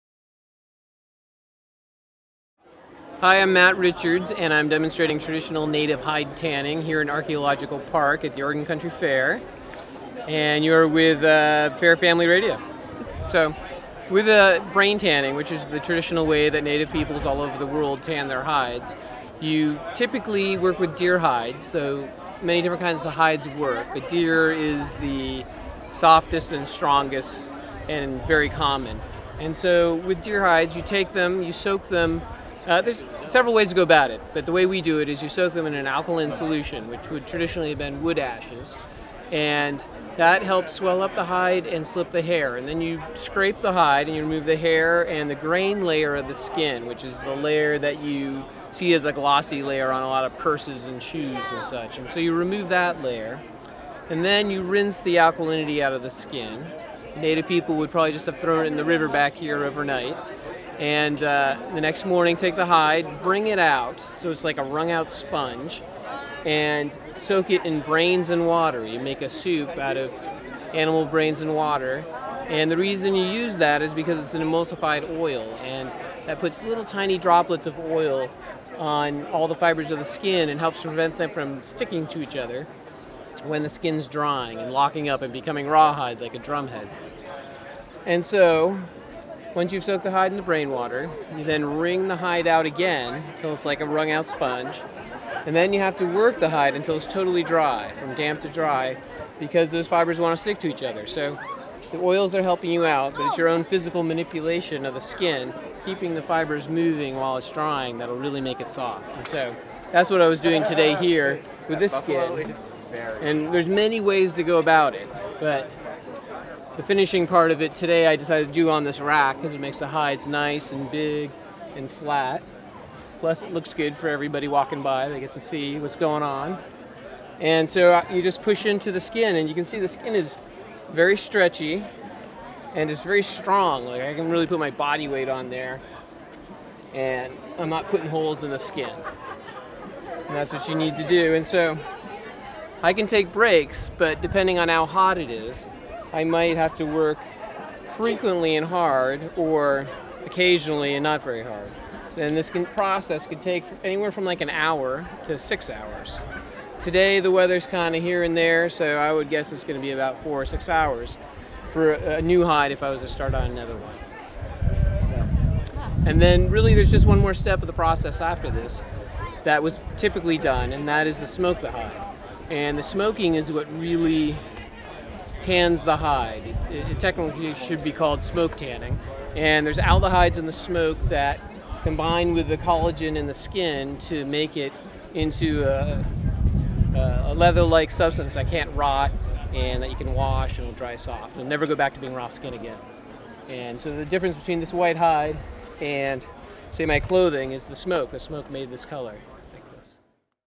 Archaeology Park - Matt Richards, LeatherTanner
Summary on how to tan a deer hide. Recorded by Fair Family Radio at the Oregon Country Fair at Archaeology Park. 2014.